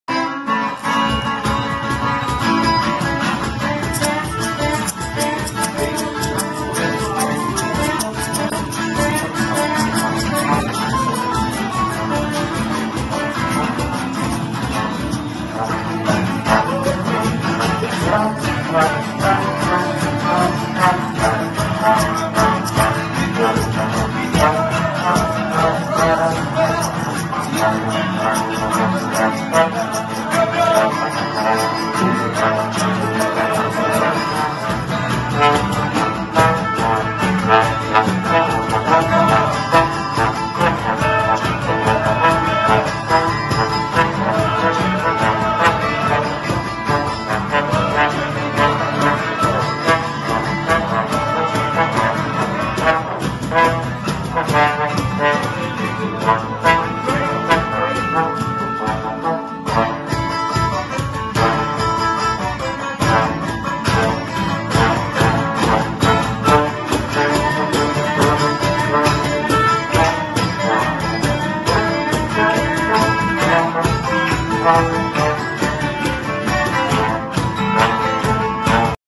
EFC 2022 November in Bavaria/Bad Kissingen/KissVino: Jam Session
One of the great parts of the English Folk Club (EFC) evening is, after weveryone has performed it can happen that a spontaneous Jam Session starts.